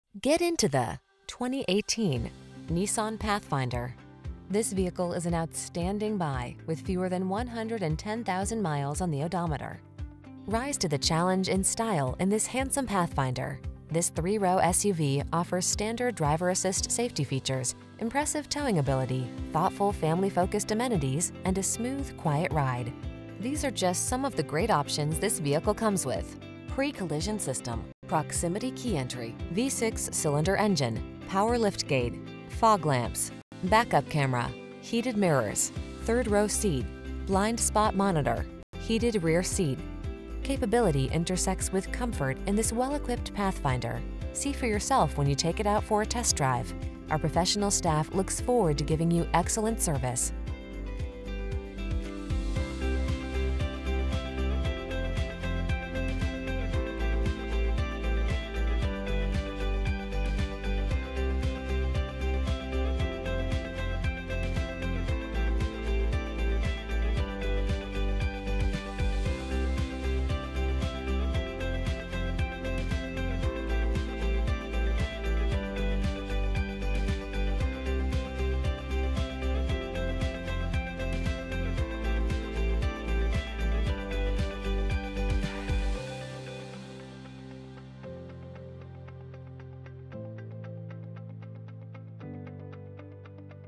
2018 Nissan Pathfinder SL TX Houston, Katy, Friendswood, Sugarland, Rosenberg
Pre-Owned 2018 Nissan Pathfinder SL available at Steele South Loop Hyundai located in 8811 Lakes at 610 Dr, Houston, TX, 77054. Servicing the Houston, Katy, Friendswood, Sugarland, Rosenberg area.
Please mention the stock JC625282P when you contact the dealership for this vehicle.